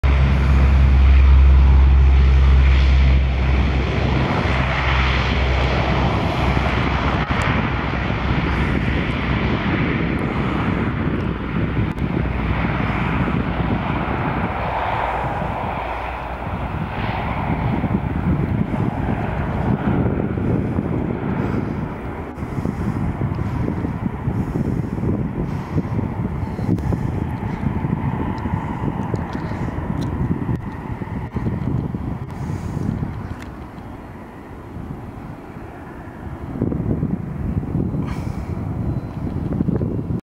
Royal Norwegian Air Force Lockheed P-3N Orion takeoff at Bremen 23.11.2014
Registration 4576 at City Airport Bremen